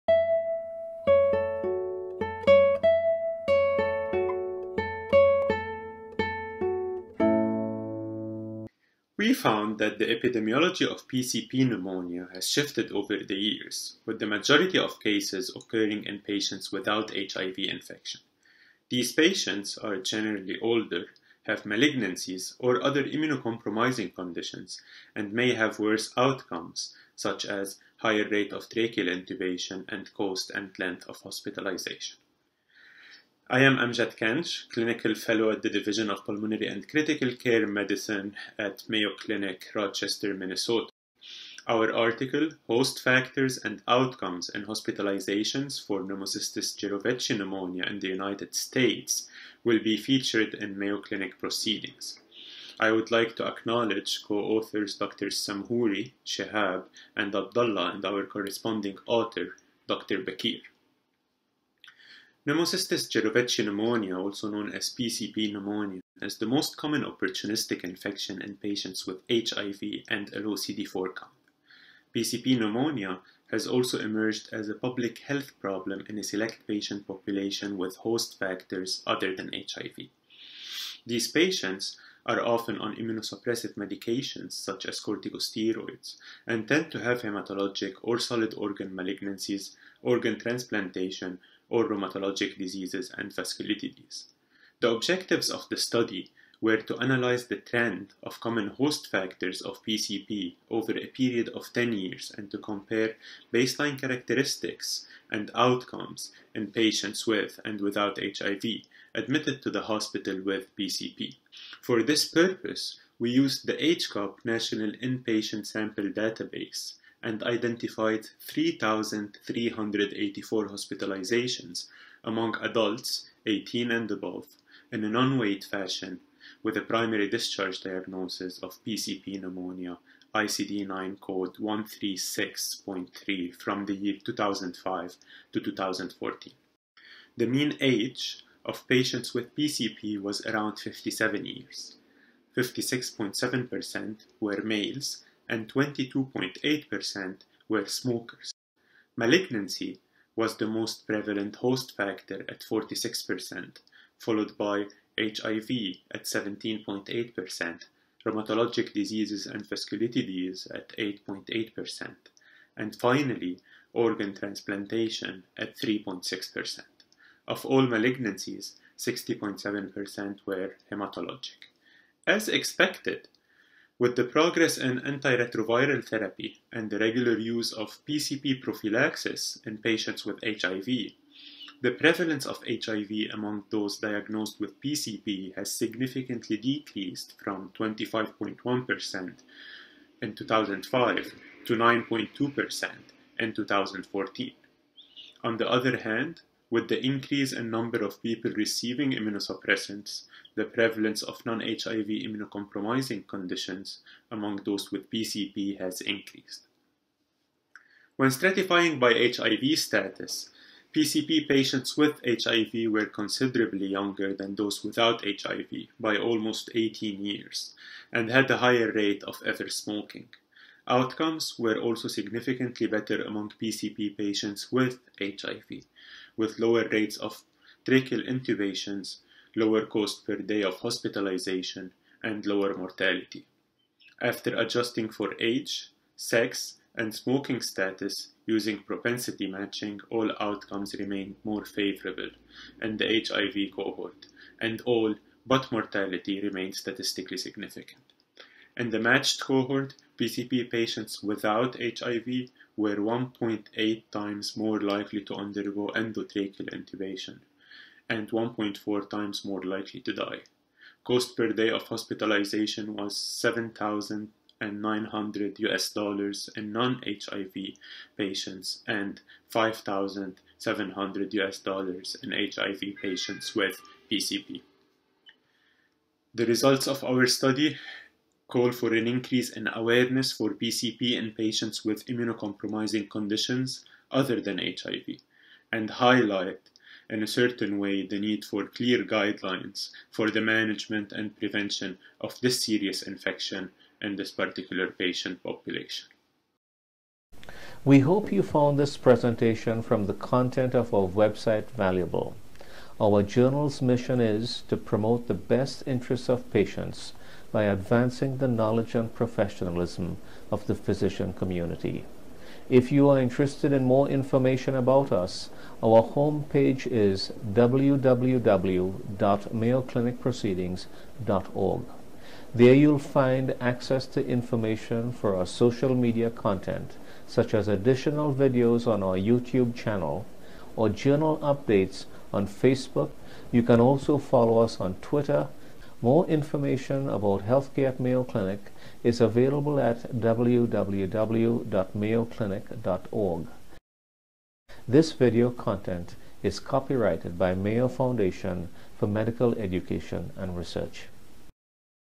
Host Factors & Outcomes for Pneumocystis Jiroveci Pneumonia in the US
Dr. Amjad Kanj, a Resident in Pulmonary and Critical Care at Mayo Clinic in Rochester, MN, reviews his article appearing in the February 2020 issue of Mayo Clinic Proceedings, where examining a 10 year time-span he noted the epidemiology of Pneumocystis Jiroveci pneumonia has shifted, with an increase in the prevalence of non-HIV patients with higher intubation rates and prolonged hospitalizations compared to matched HIV patients.